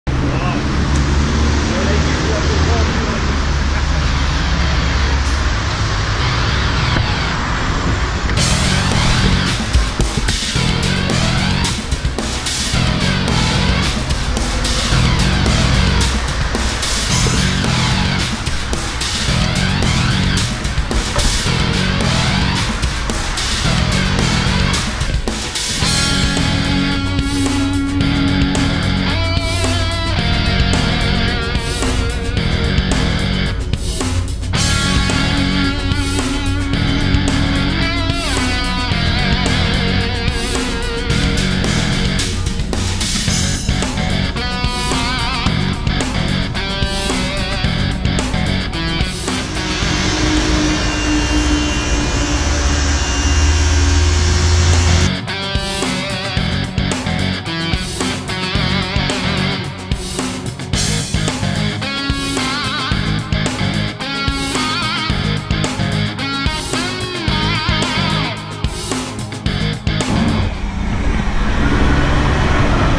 Der Ford Cargo kommt nach Europa
Der türkisch-amerikanische Hersteller Ford Otosan nimmt mit seinen neuen Euro-6-Fahrzeugen Europa in den Fokus. Ein gewagter Schritt – trotz durchaus harmonischer Trucks. lastauto omnibus hat ihn in einem Fahrbericht für Ausgabe 7/2017 unter die Lupe genommen.